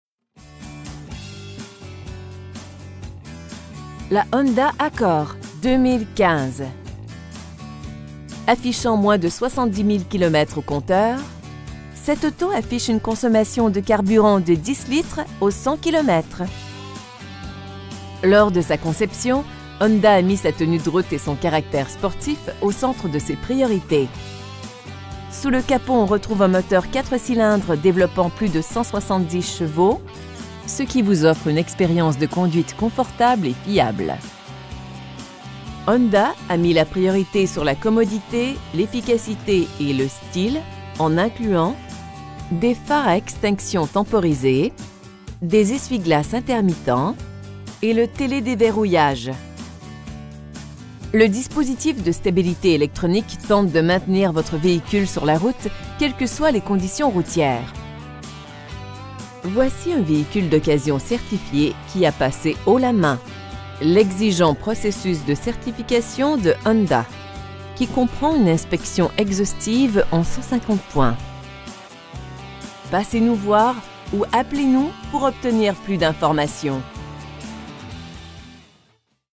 2015 Honda Accord EX TOIT/CAMERA
Honda Gabriel
7000, Henri-Bourassa Est
in Montréal Nord, QC H1G 6C4

Attendez-vous toujours à plus avec la Honda Accord 2015. Affichant moins de 70 000 km au compteur, cette auto affiche une consommation de carburant de 10 litres aux cent kilomètres. Lors de sa conception, honda a mis sa tenue de route et son caractère sportif au centre de ses priorités. Sous le capot on retrouve un moteur 4 cylindres développant plus de 170 chevaux, ce qui vous offre une expérience de conduite confortable et fiable. Honda a mis la priorité sur la commodité, l'efficacité et le style en incluant: des phares à extinction temporisée, l'ouverture automatique de la glace du conducteur, un compte-tours, des essuie-glaces intermittents, un ordinateur de bord, rétroviseurs extérieurs chauffants, et le télédéverrouillage. Là où il fait plus froid, les occupants apprécieront à leur juste valeur les sièges chauffants et le dispositif de climatisation contrôlée. Ses élégantes roues en alliage contribuent à son efficacité dynamique. Le toit ouvrant à commande électrique permet de se rapprocher de la nature et d'humer l'air frais en un tour de main. La chaîne haute fidélité alimente 7 haut-parleurs, qui remplissent l'habitacle de voluptueuses ondes auditives. Ce véhicule comporte de nombreuses caractéristiques de sécurité: rideaux gonflables pour protéger la tête, coussins gonflables latéraux à l'avant, système antipatinage, système d'assistance au freinage, un avertisseur d'urgence, et freins antiblocage. Le dispositif de stabilité électronique tente de maintenir votre véhicule sur la route quelles que soient les conditions routières. Voici un véhicule d'occasion certifié qui a passé haut la main l'exigent processus de certification de Honda, qui comprend une inspection exhaustive en 150 points! Notre fierté se manifeste à travers l'excellent service à la clientèle que nous offrons. Passez nous voir ou appelez-nous pour obtenir plus d'information.